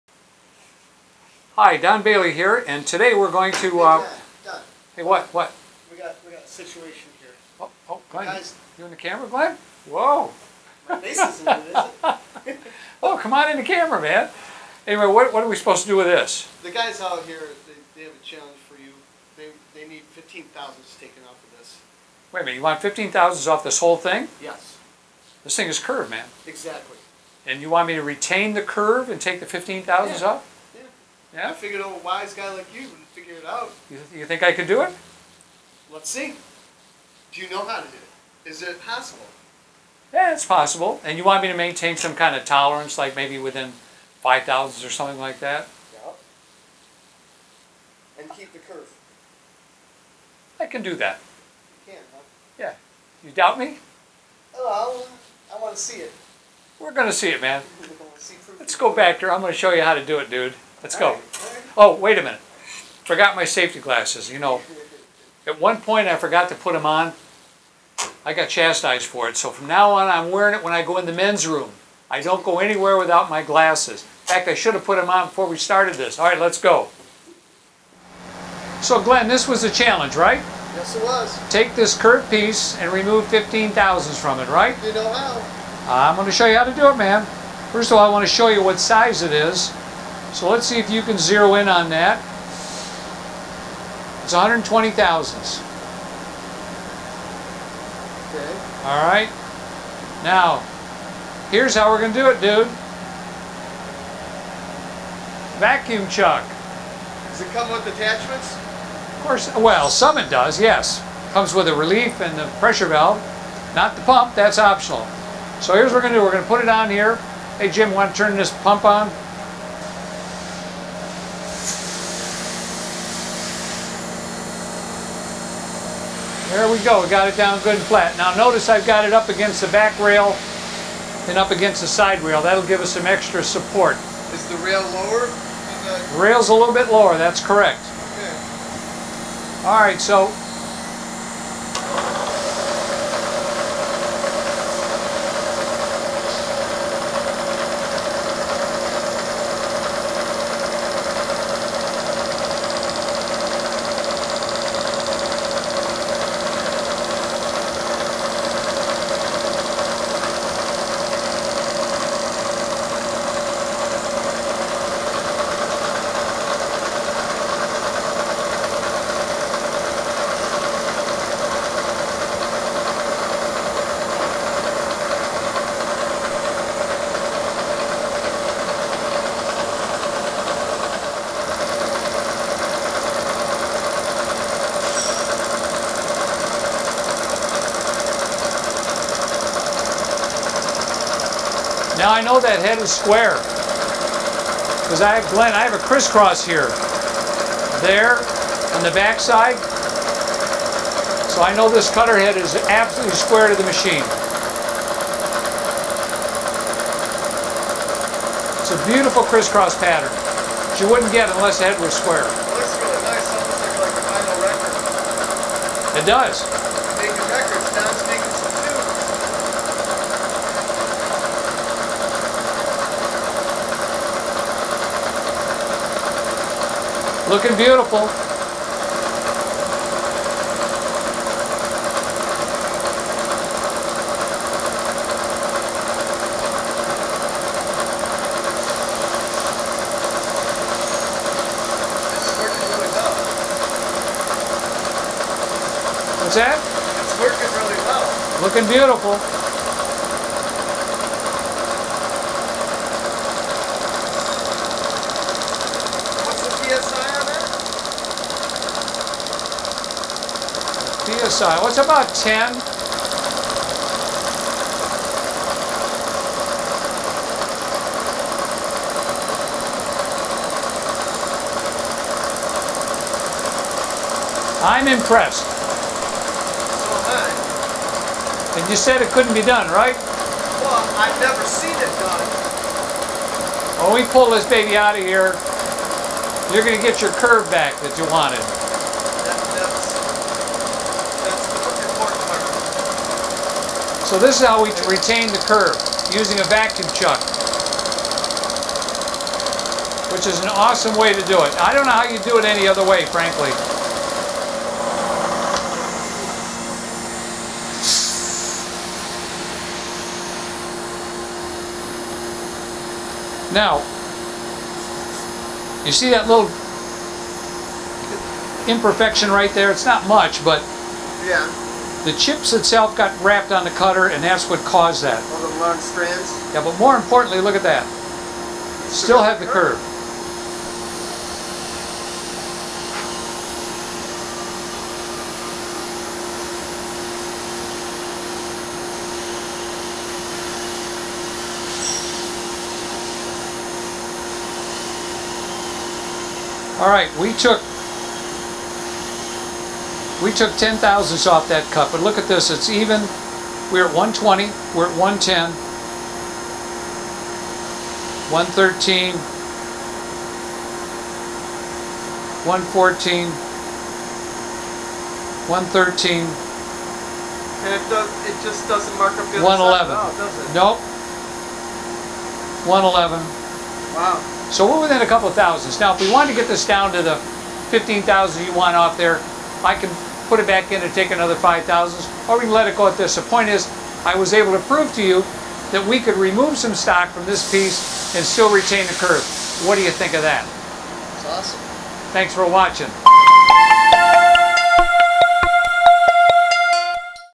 Don Bailey Shows How to Hold Thin Plastic When Milling on a Bridgeport
Don Bailey demonstrates how to mill thin non ferrous materials on a Bridgeport with a Suburban Tool Vacuum Chuck.